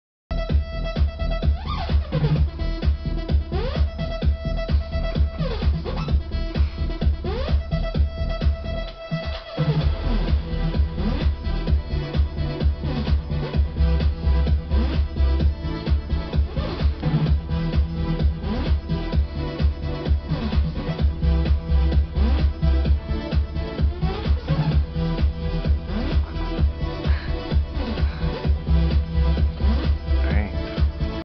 Dancing Beagle Puppy!
just a cute and funny video of my brother holding my now 9 week old beagle puppy and making him dance. enjoy!